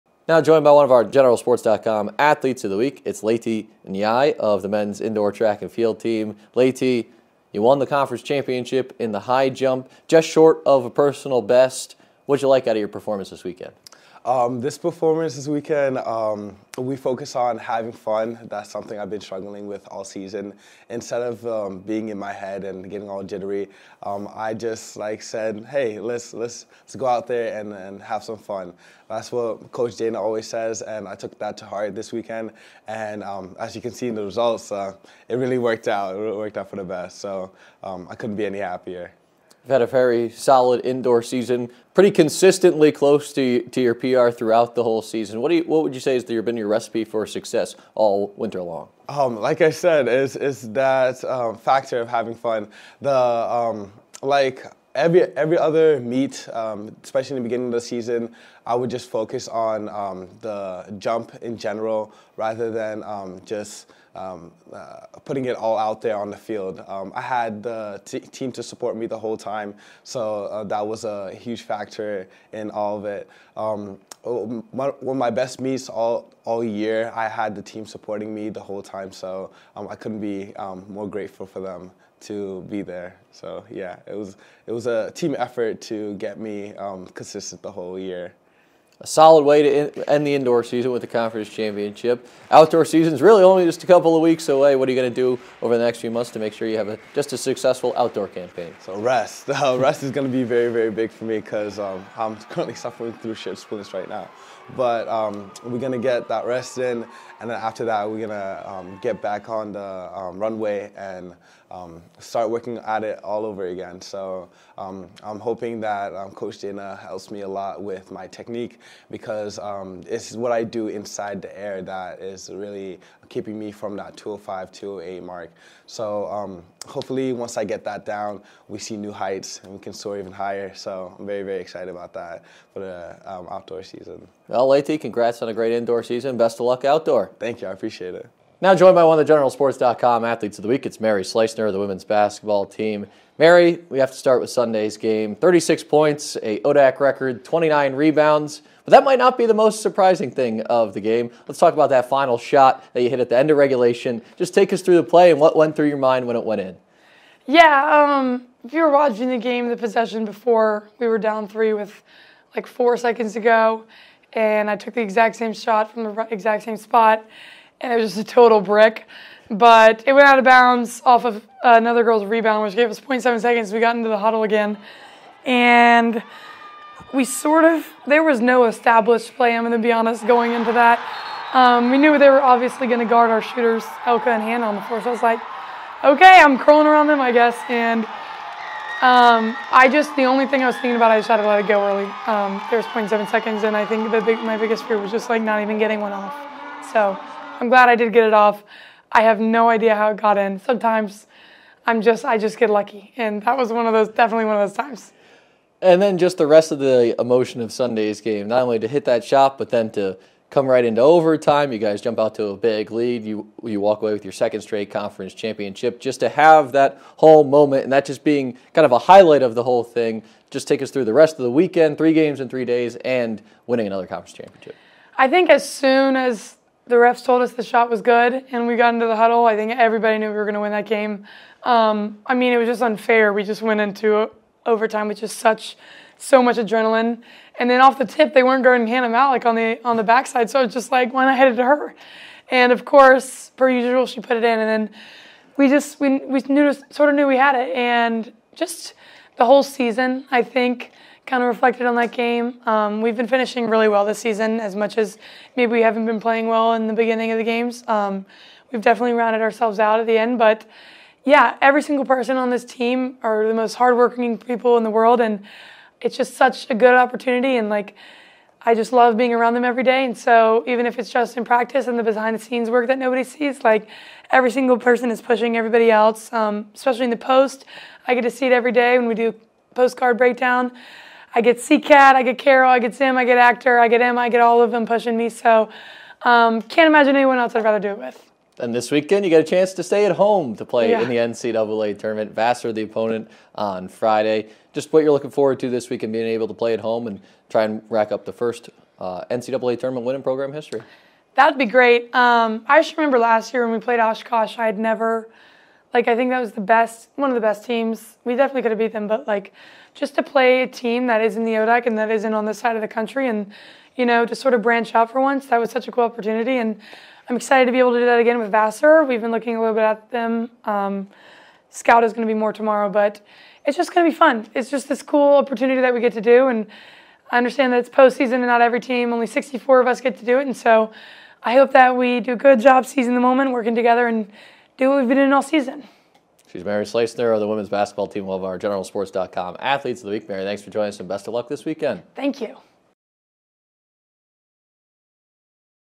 Generalssports.com Athletes of the Week (2/28/24)
Leyti Ndiaye of the Washington and Lee men's track & field team won the ODAC Championship in the High Jump, while Mary Schleusner helped power W&L women's basketball to its second straight ODAC Championship with a record-breaking weekend in Salem!